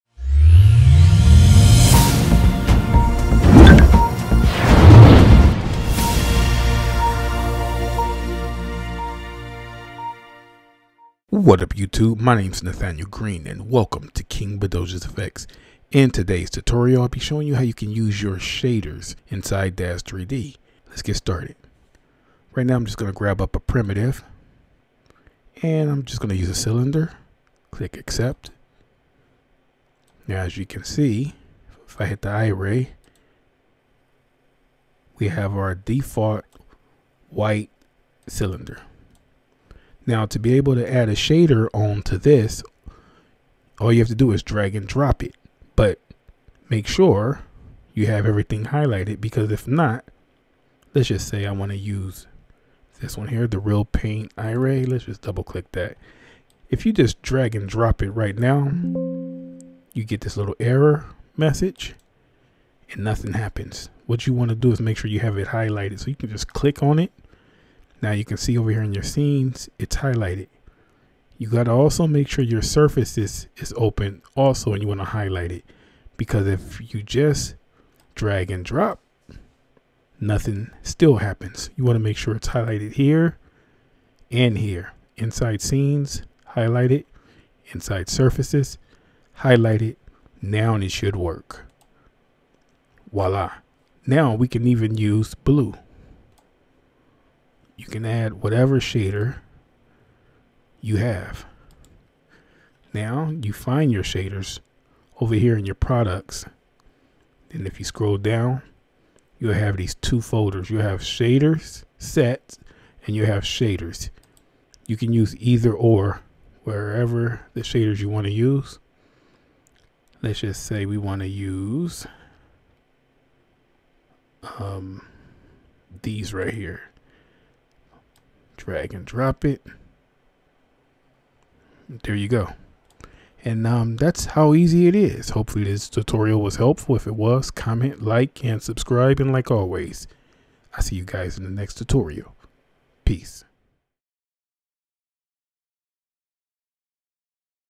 🔴 The ULTIMATE Daz3D Shaders Tutorial (2024)
In this 2-minute tutorial, I will show you how to start using your shaders in Daz3D like a pro. Have a great day and get creative!